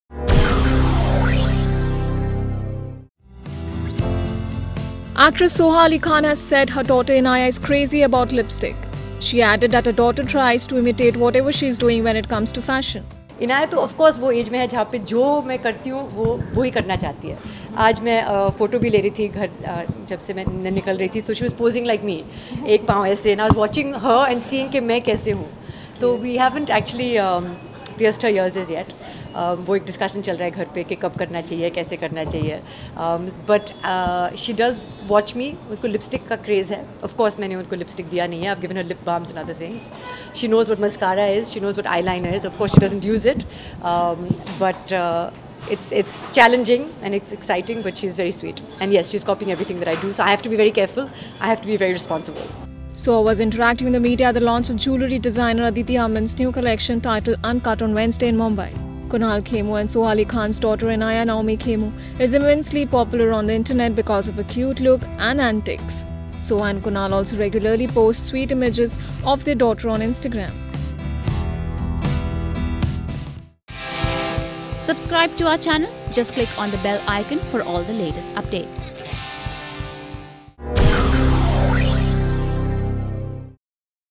Inaaya crazy about lipstick; copies everything I do: Soha Ali Khan
Actress Soha Ali Khan has said her daughter Inaaya is crazy about lipstick. She added that her daughter tries to imitate whatever she is doing when it comes to fashion.